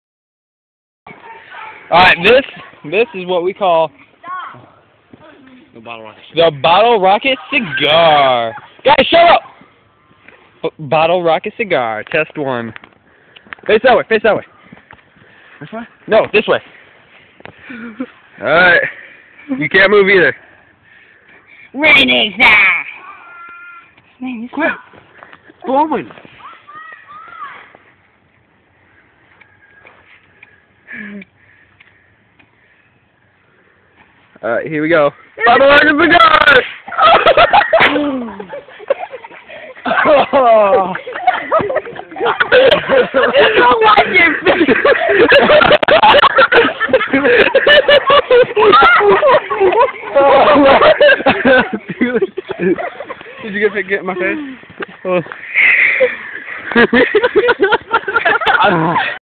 Bottle rocket cigar
My bro shoots a bottle rocket out of his mouth